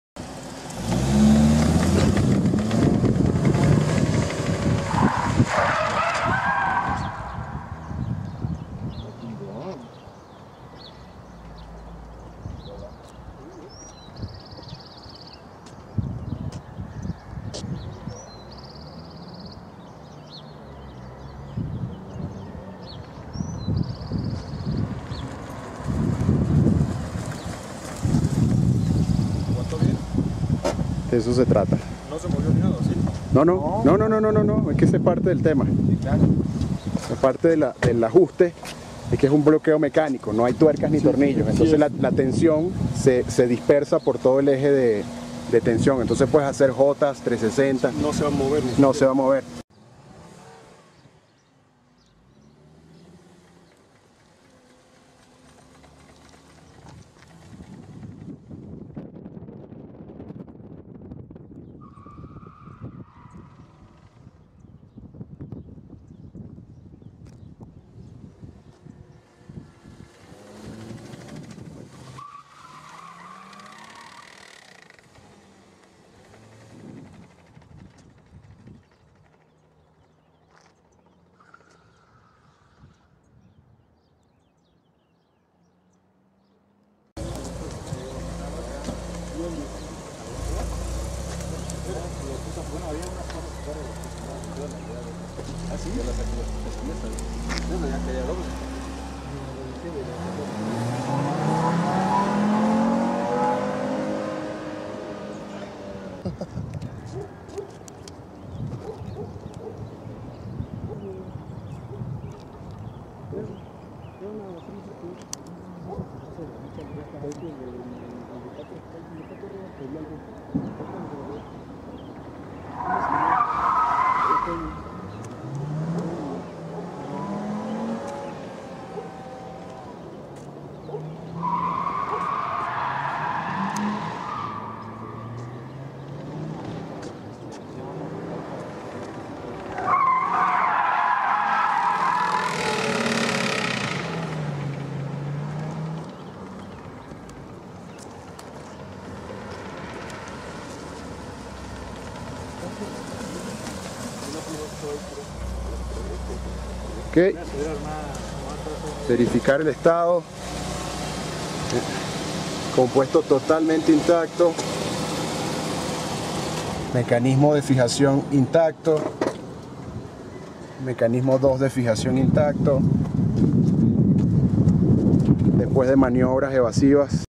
Flats Over: Evasive Driving - Mexico DF 2015
(En) Testing the exceptional capabilities of Flats Over under real world evasive manoeuvres. Most of attacks on armoured vehicles target tires and windshield. Drivers must be capable to perform emergency driving manoeuvres with unstructured tires. In some cases without tire tread, and with limited vision due to bullets impacts in the windshield. With our partner Prorescue MX. Mexico City – 2015

(P) Testando as capacidades excepcionais do Sistema Flats Over em condições reais de dirigibilidade. A maioria dos ataques a veículos blindados são direcionados ao para-brisas e as rodas.  Condutores devem poder realizar manobras de evasão emergenciais até com pneus desestruturados.  Em alguns casos mesmo sem banda de rodagem e com visão comprometida devido a impactos de projeteis no para-brisas. Realizado em conjunto com nosso parceiro local Prorescue MX. Cidade do México, 2015.

(Es) Prueba de las capacidades excepcionales de Flats Over bajo maniobras evasivas del mundo real. La mayoría de los ataques contra vehículos blindados se dirigen a los neumáticos y parabrisas. Los conductores deben ser capaces de realizar maniobras de conducción de emergencia con los neumáticos no estructurados. En algunos casos, sin la banda de rodadura de los neumáticos, y con visión limitada, debido a los disparos en el parabrisas. Con nuestro socio Prorescue MX. Ciudad de México - 2015